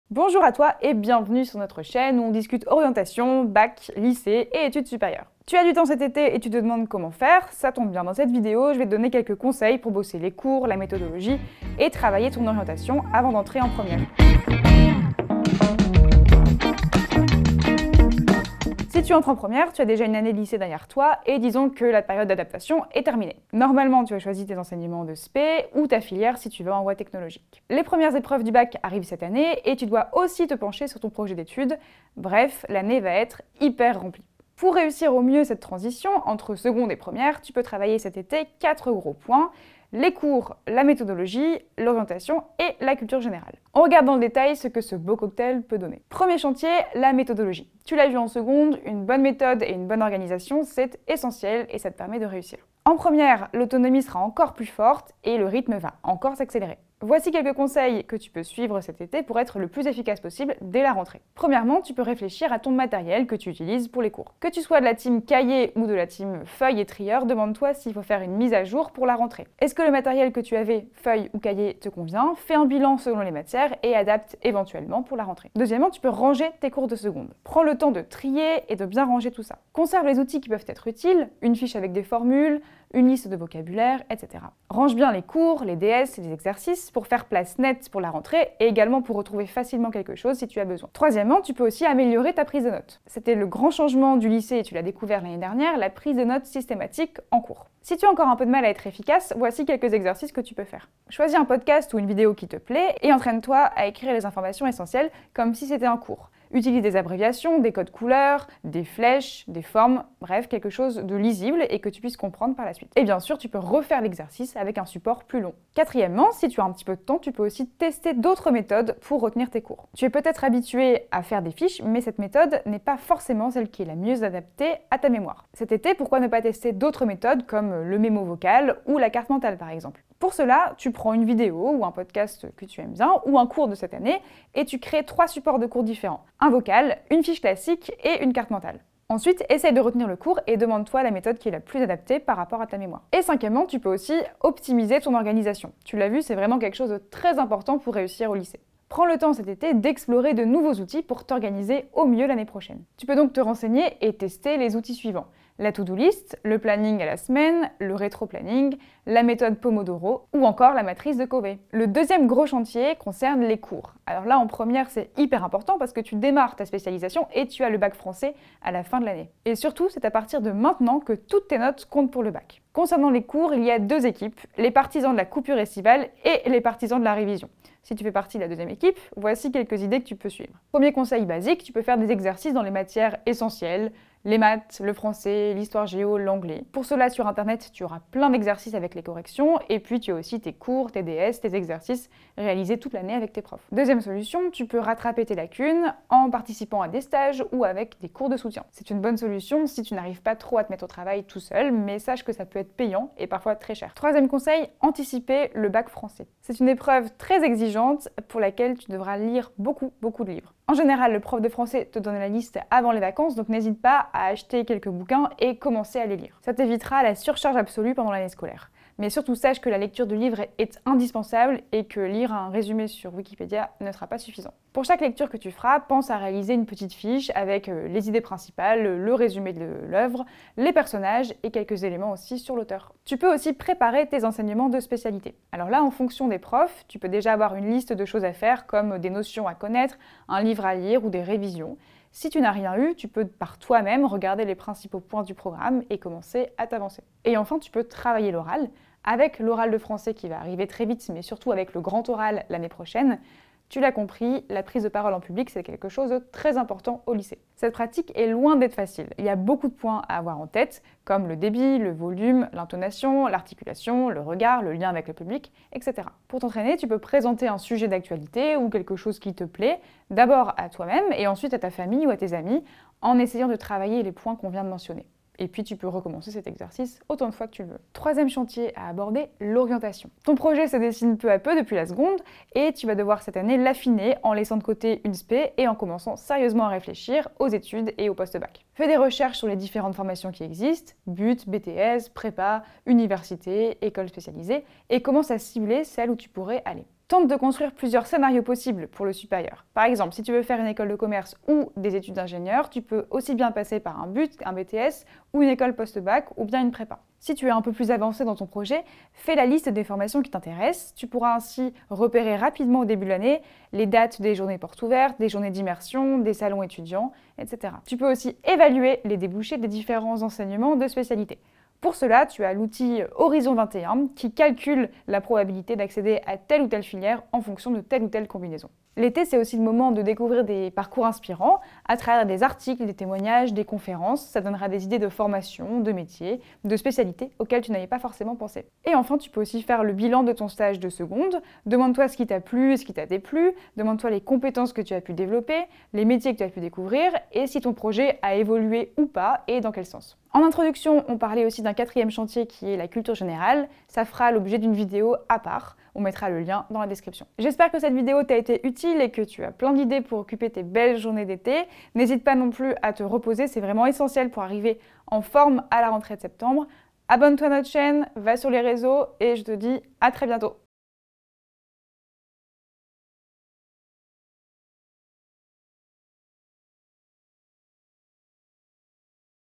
Optimise ton été avant la première ! 💪
Tu entres en première en septembre ? Les choses sérieuses continuent ! Prends le temps cet été de préparer cette nouvelle rentrée au lycée ! LIRE LA SUITE 👇👇👇

😍 Dans cette vidéo "Optimise ton été avant la première ! 💪" 📚

Tu es au clair avec le fonctionnement du lycée puisque tu as eu toute ton année de seconde pour t'y adapter. Place maintenant à une année importante puisqu'en première :
1️⃣ tu vas entamer l'étude de tes enseignements de spécialités ;
2️⃣ tes notes vont compter pour le bac ;
3️⃣ les épreuves anticipées du bac de français ont lieu à la fin de l'année...
Voici nos conseils pour débuter ton année de première dans les meilleures conditions !

🚨LES PARTIES DE LA VIDÉO "Optimise ton été avant la première ! 💪" 🚨

0:00​ • Introduction
0:46 • Organisation & méthodo
2:53 • Révise tes cours
3:24 • Zoom sur le bac français
3:55 • Les enseignements de spécialité
4:06 • Travailler l'oral
4:39 • Ton projet d'orientation

📚 BAC FRANÇAIS 2023
La liste des œuvres au programme

Nos vidéos avec Florine :
☀️ Nos conseils pour booster ta culture gé pendant l'été ⏯